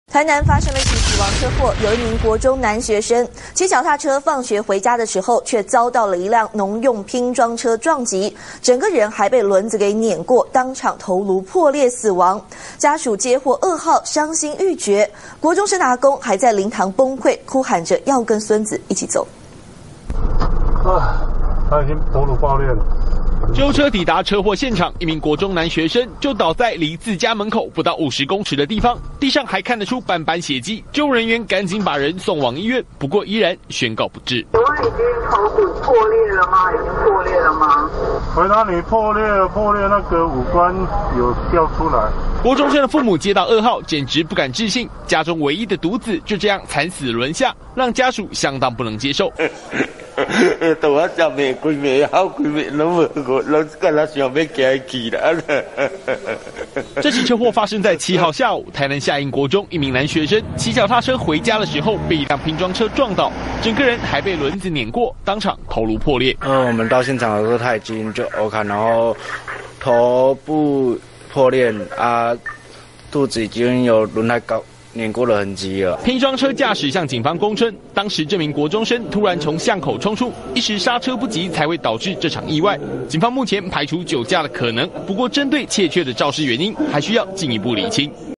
中視新聞》可惡酒駕！農用拼裝車撞死鐵馬國中生
中視新聞》台南日前發生一起死亡車禍，一名國中男學生騎腳踏車放學回家時，遭到一輛農用拼裝車撞擊，整個人還被輪子輾過，當場頭顱破裂死亡，家屬接獲噩耗傷心欲絕，國中生的阿公還在靈堂崩潰，哭喊要跟孫子一起去。